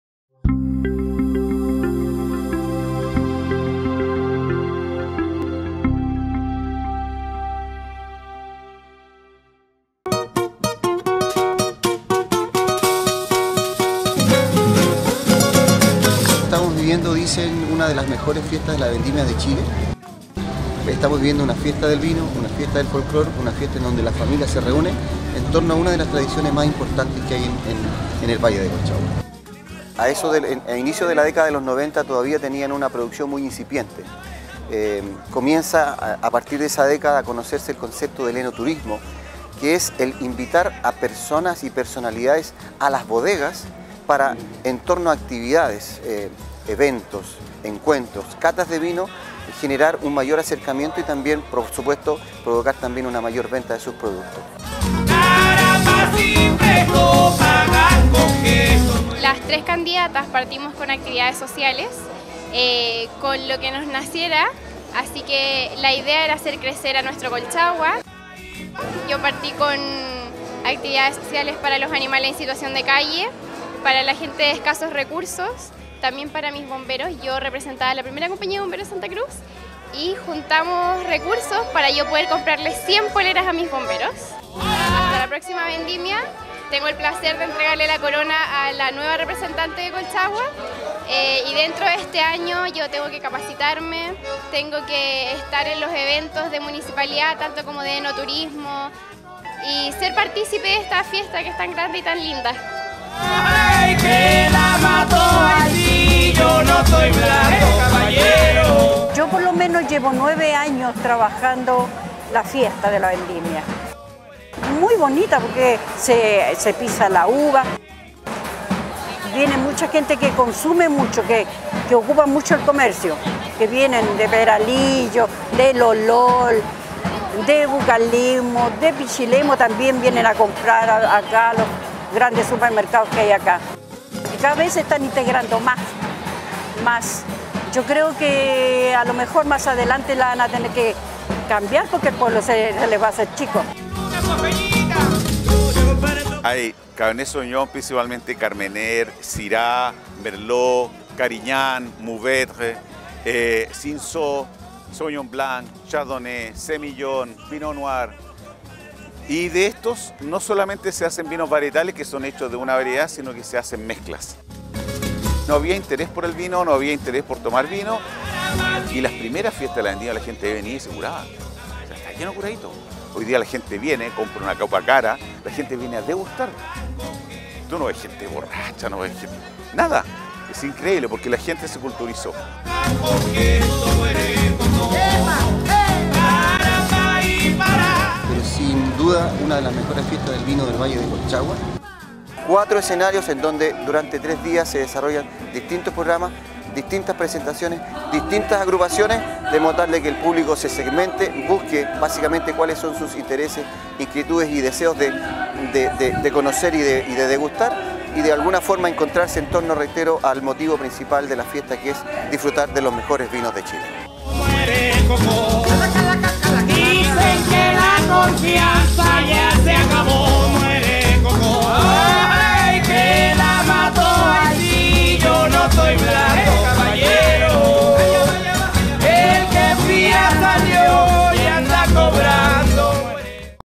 A 20 años de la primera Fiesta de la Vendimia de Santa Cruz
20 años se cumplieron de la primera Fiesta de la Vendimia de Santa Cruz, que ha tenido siempre por objetivo que la comunidad pueda conocer esta antigua tradición y degustar de los mejores vinos de Chile. Durante tres días, fueron diferentes bodegas o empresas vinícolas, las  que pudieron ofrecer sus productos, junto con shows musicales y comida típica.